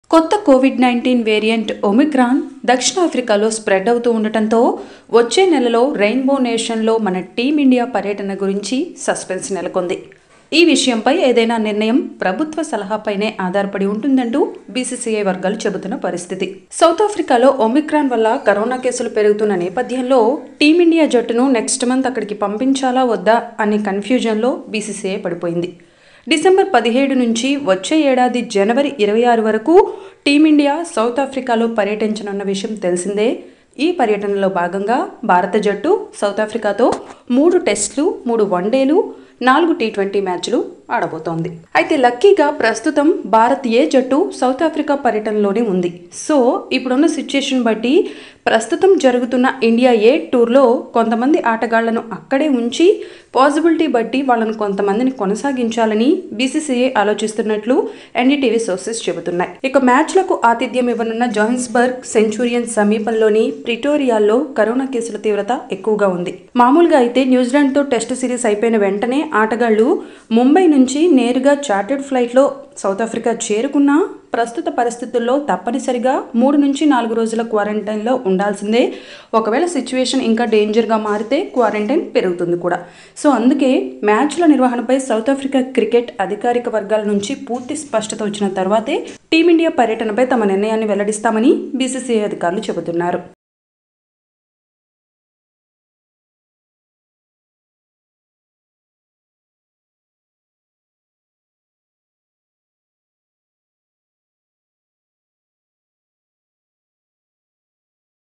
IND VS SA Series In Doubtful | COVID Omicron Variant|India Tour Of SA || Oneindia Telugu
India Tour Of South Africa 2021 in dilama amid New COVID Variant concern in South Africa.

టీమిండియా సౌతాఫ్రికా పర్యటన రద్దు అయ్యే అవకాశం కన్పిస్తోంది. కరోనా కొత్త వేరియెంట్ సౌతాఫ్రికా లో స్ప్రెడ్ అవుతున్న నేపథ్యంలో ఈ సిరీస్ మీద ఇంకా ఎటువంటి నిర్ణయం తీసుకోలేదు బీసీసీఐ. మాములుగా అయితే డిసెంబరు 17 నుంచి వచ్చే ఏడాది జనవరి 26 వరకు టీమిండియా.. సౌతాఫ్రికాలో పర్యటించాల్సి ఉంది.  కానీ ఏం జరుగుతుందో తెలియని పరిస్థితి
Oneindia Telugu